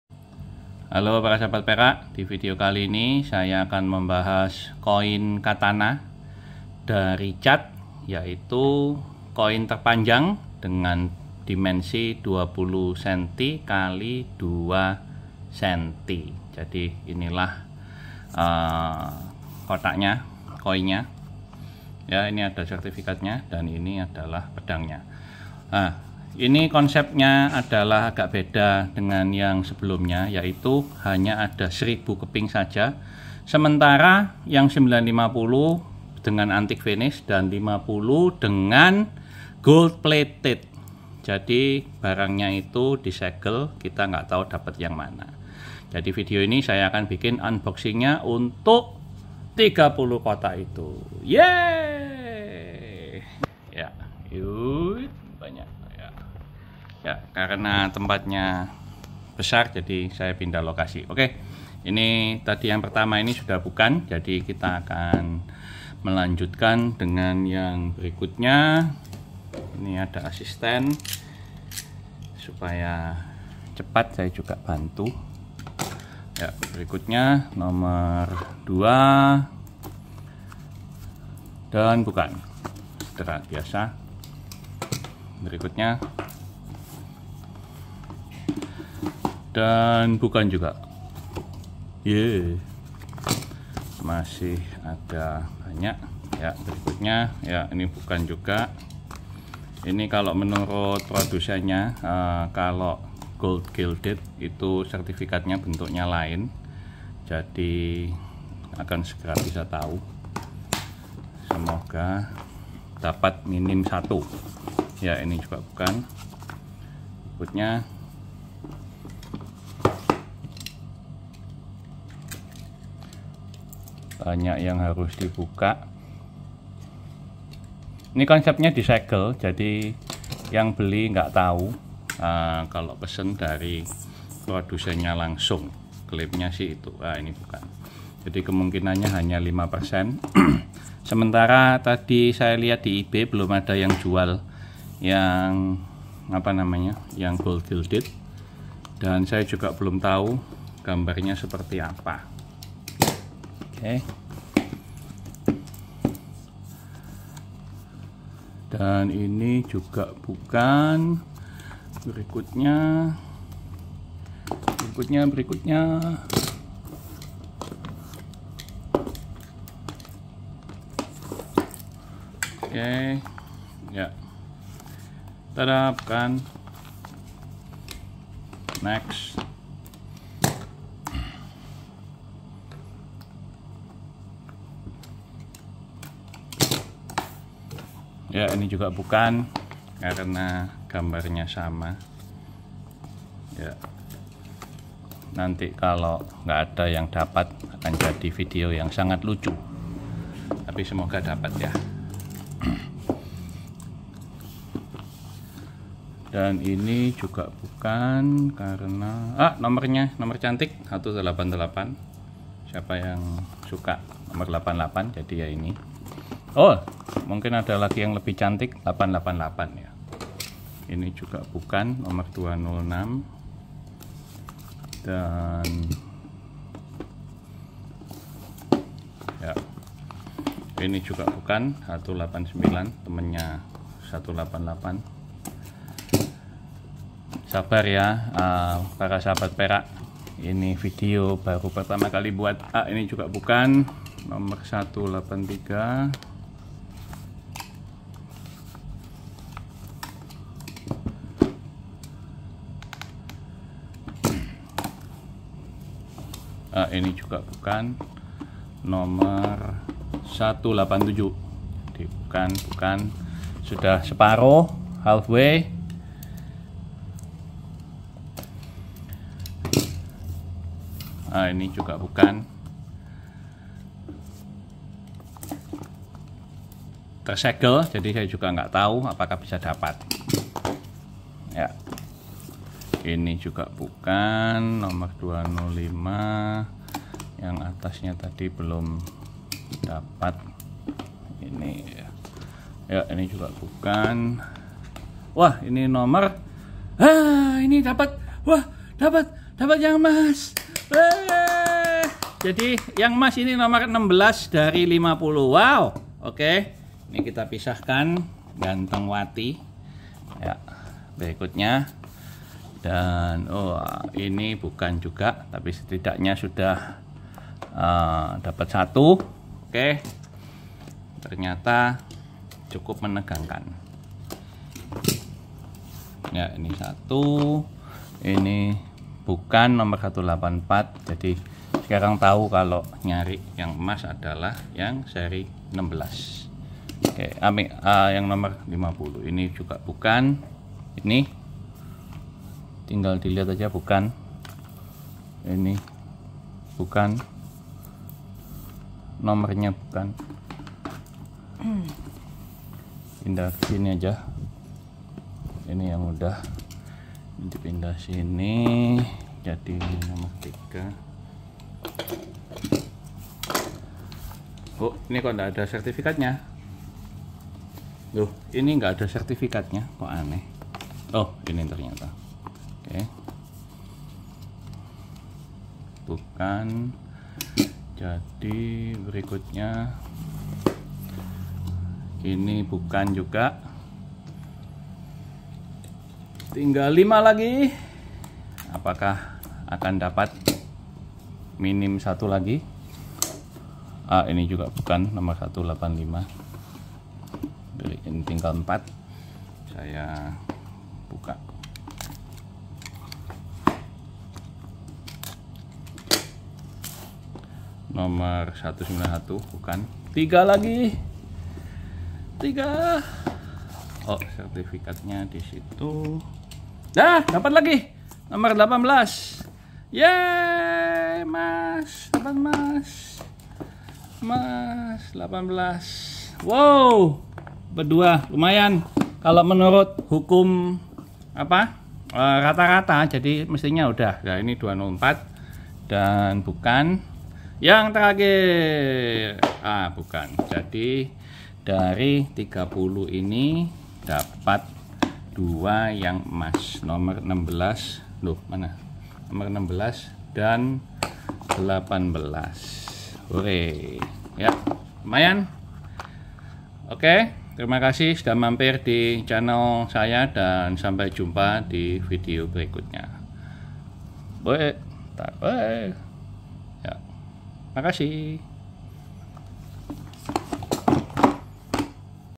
Unboxing Koin Katana 2oz Silver dari Chad 2024
Proses unboxing mencari edisi spesial gold plated Katana dari Chad 2024. Dari 1000 keping mintage yang ada 950 difinish Antique dan 50 finish gold plated.